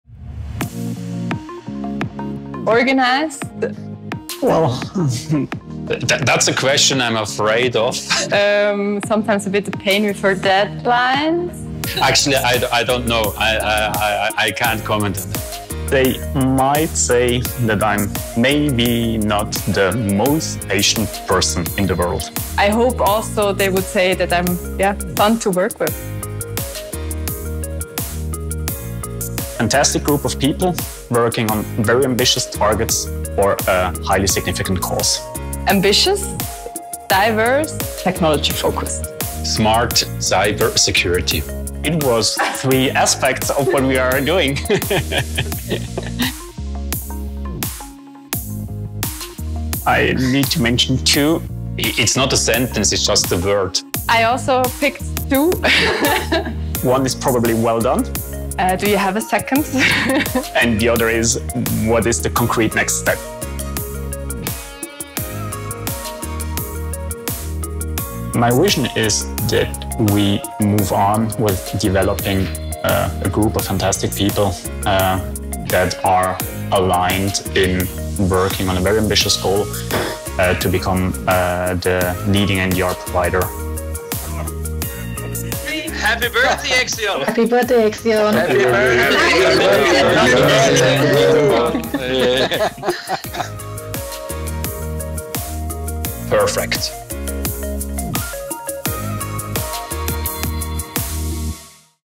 Happy 8th Birthday, Exeon! — Management Interviews
As we celebrate our company's eighth birthday, it's a perfect time to reflect on the journey we've taken together at Exeon. To mark this special occasion, we posed one question to each member of our management team, diving into their personal experiences, challenges faced, and vision for the future. Join us as we delve into their unique insights and celebrate the milestones we've achieved together. Happy birthday, Exeon!